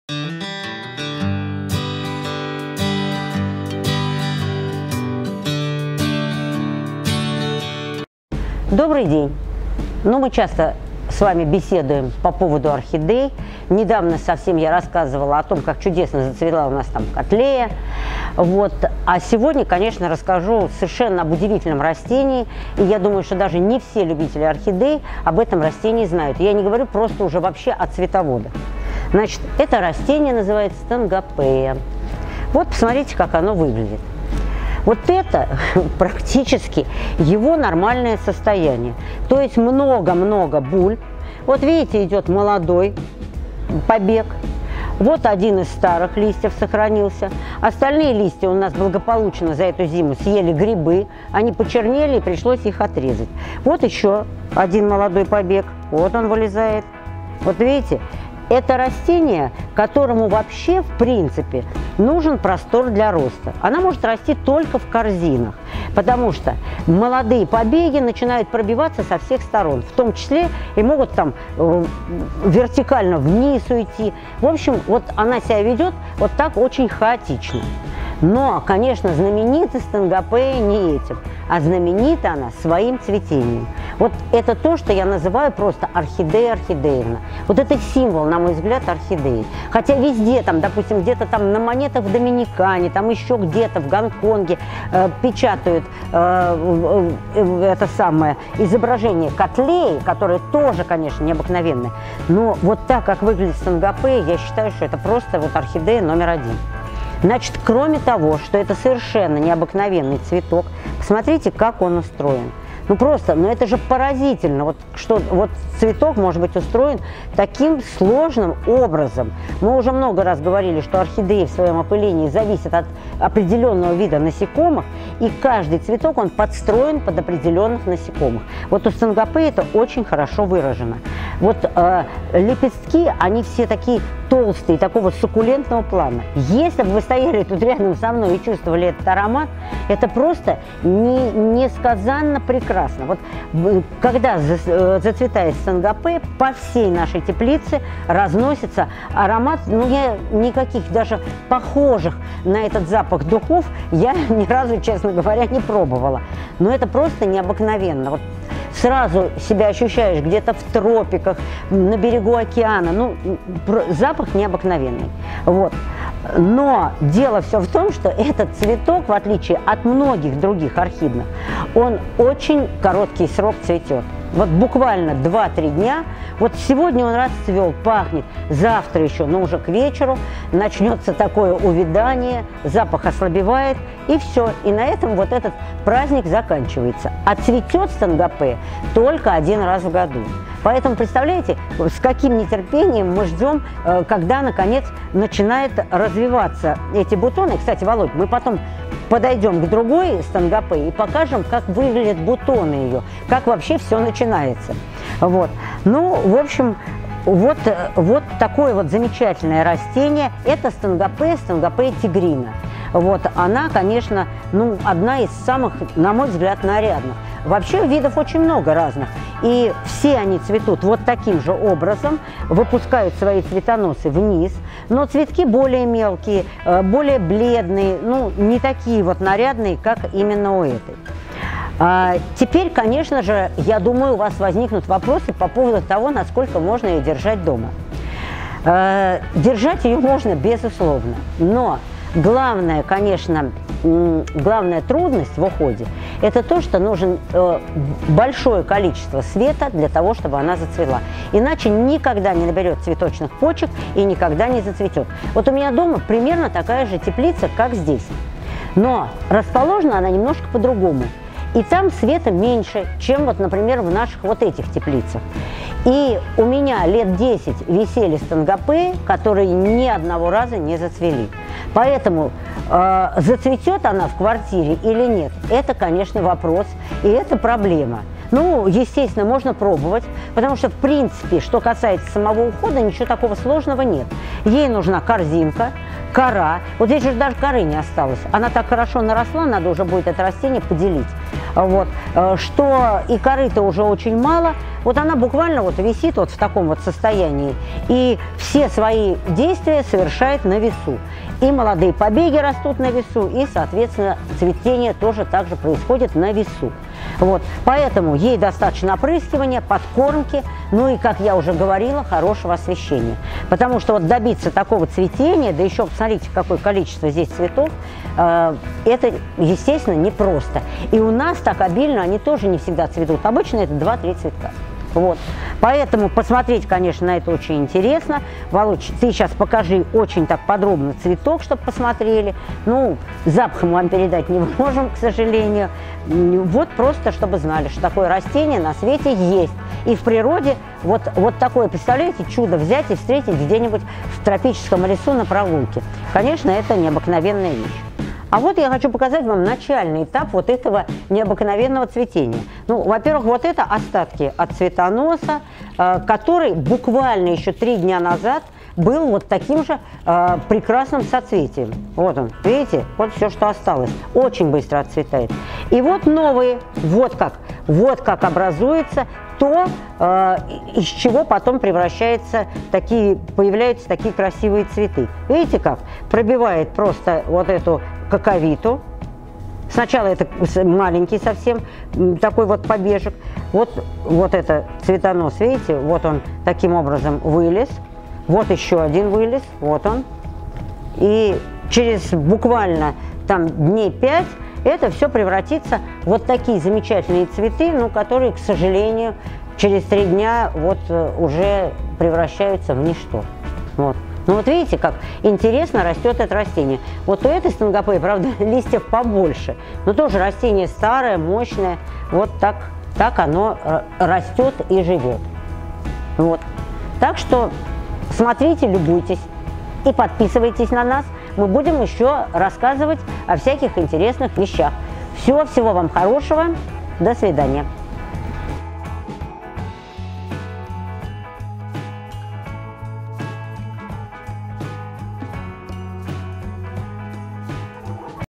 Стангопея тигровая.Аромат этой орхидеи словами не передать.
Стангопея (лат. Stanhopea) — большой и довольно популярный род эпифитных орхидей, распространенный в дождевых тропических лесах от Мексики до Бразилии.Мы поговорим о Стангопея тигрина.